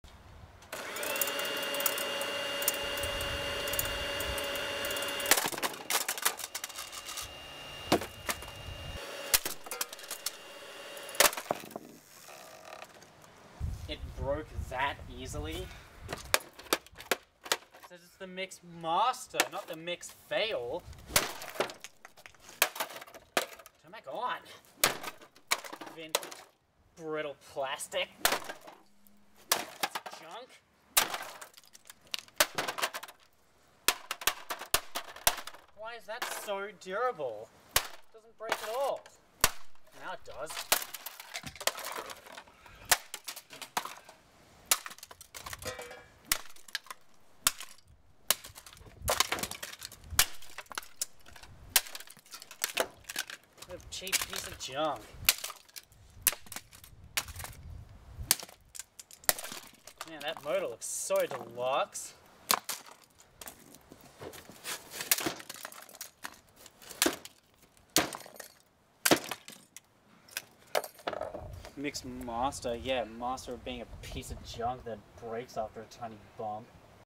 Vintage Sunbeam Mixmaster Destruction
It wasn't the master of mixing because it broke so easily!

Model: Sunbeam Mixmaster MX-AB mixer.